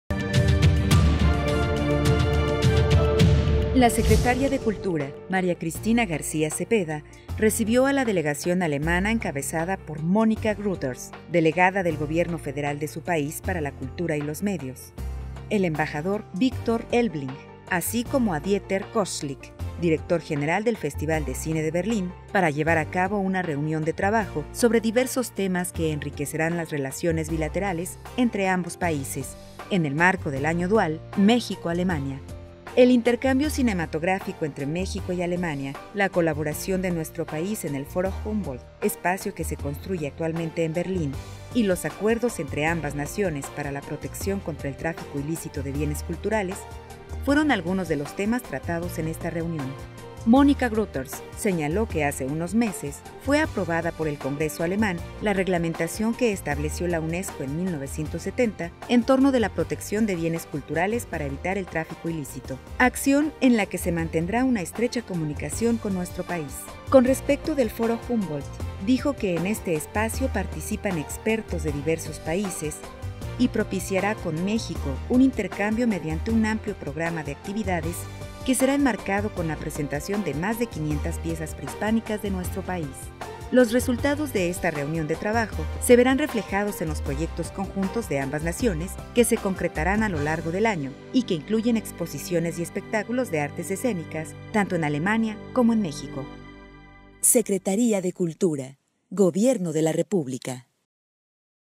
Reunión de Trabajo secretaria de Cultura y delegación alemana
La secretaria de Cultura, María Cristina García Cepeda, recibió a la delegación alemana encabezada por Monika Grütters, delegada del gobierno  federal de su país para la Cultura y los Medios; el embajador Viktor Elbling, así como a Dieter Kossslick, director general del Festival de Cine de Berlín, para llevar a cabo una reunión de trabajo sobre diversos temas que enriquecerán las relaciones bilaterales entre ambos países en el marco del Año Dual México-Alemania